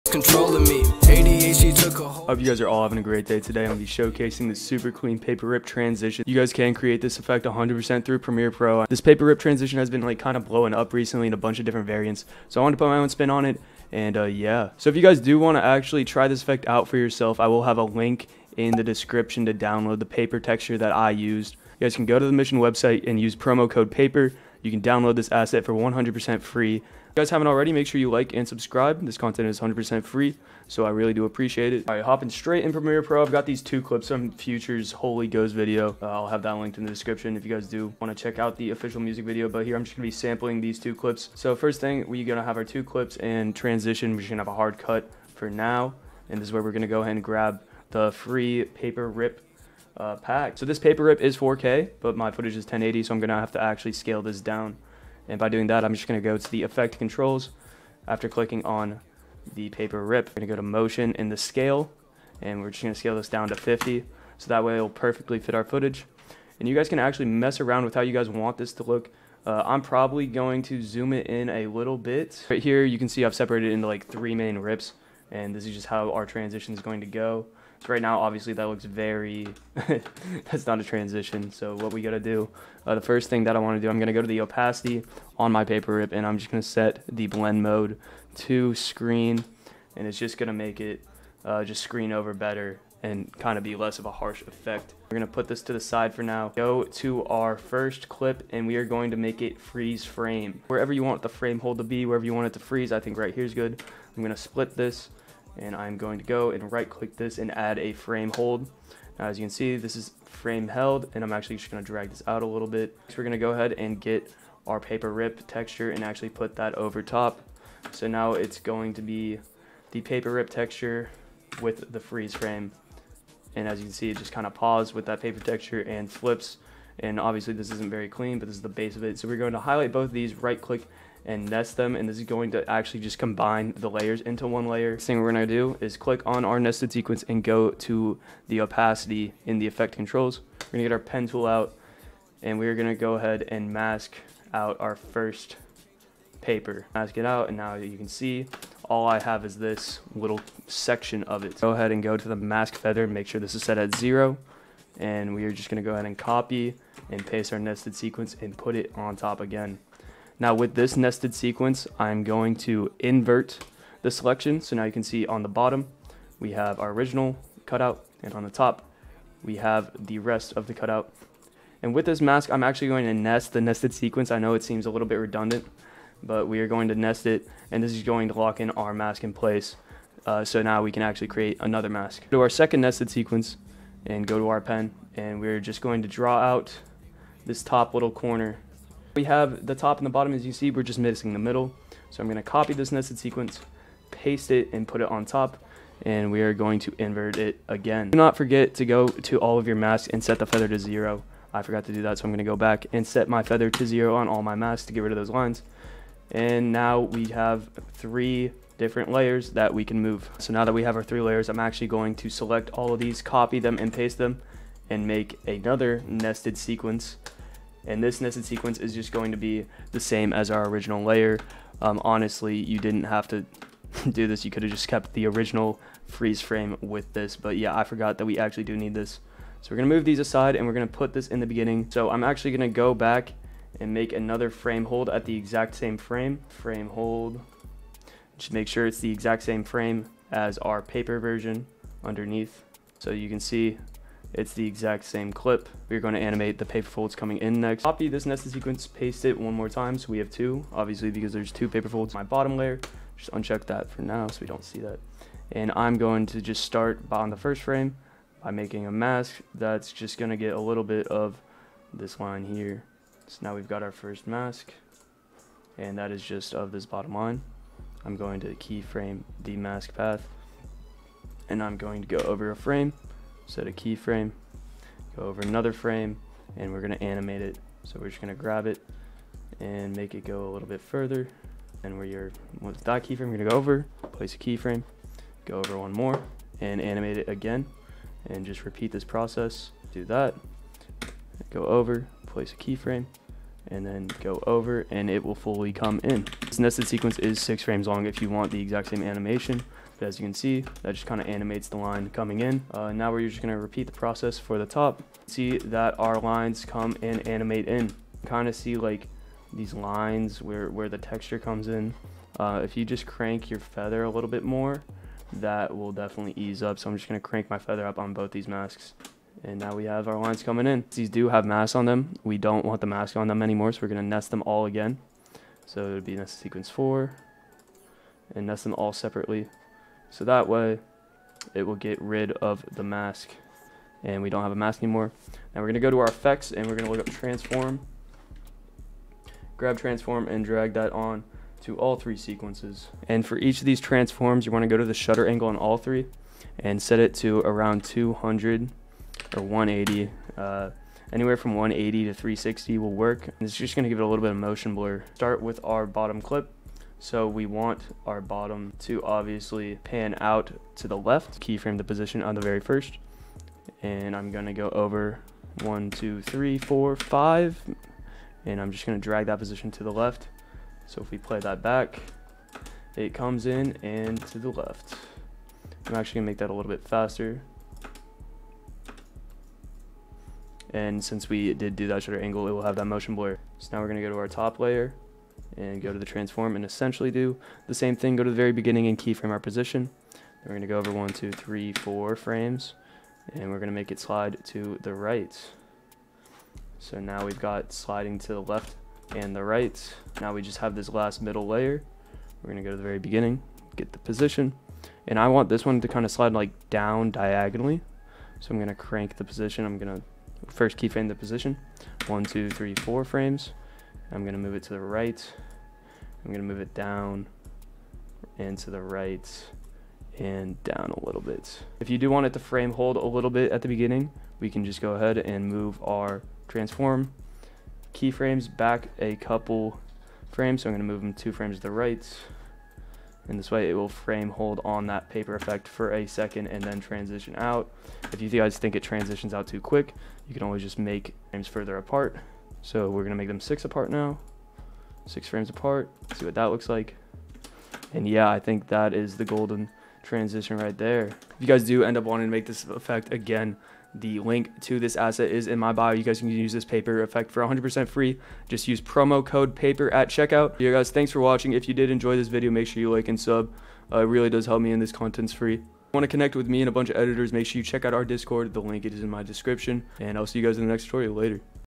add INSANE PAPER TRANSITIONS to your MUSIC VIDEOS [Premiere Pro]
DOWNLOAD YOUR FREE PAPER OVERLAY :
Code : "PAPER" for 100% off (for free)

0:00 Intro
0:32 Frame Hold & Overlay
2:15 Masking Paper Sections
4:12 Animating Paper Rips
7:02 Keyframe Timing